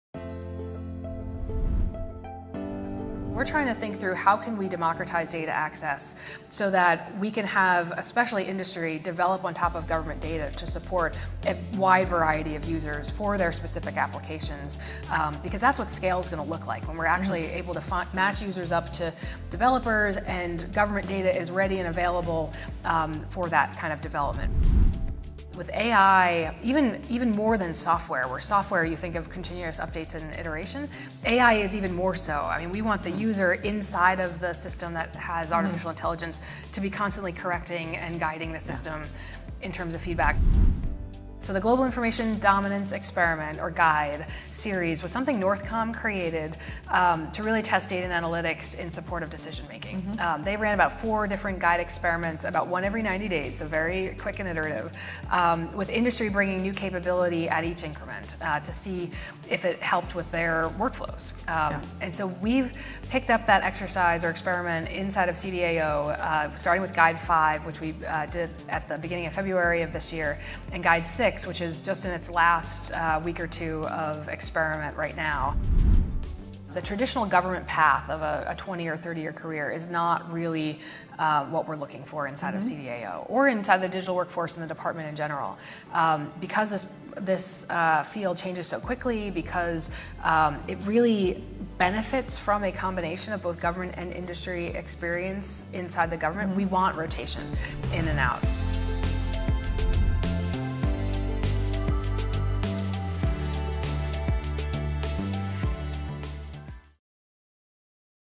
The ReCap | Acquisition for Decision Advantage
In 2022, the Department of Defense (DoD) stood up the Chief Digital and Artificial Intelligence Office (CDAO) with the goal of generating decision advantage by accelerating DoD’s adoption of data, analytics, and artificial intelligence. CSIS’s Defense-Industrial Initiatives Group hosted a discussion between Dr. Cynthia R. Cook and Deputy CDAO Margie Palmieri to discuss the challenges DoD faces in acquiring and scaling the latest technology to enable digital and AI-enabled solutions, especially where DoD processes do not align with the speed of technological change.